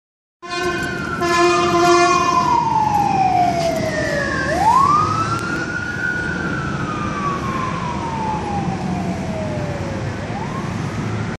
LAFD Rescue 881 Responding
Rescue 881 responding to a medical call with Light Force 39 and Rescue 39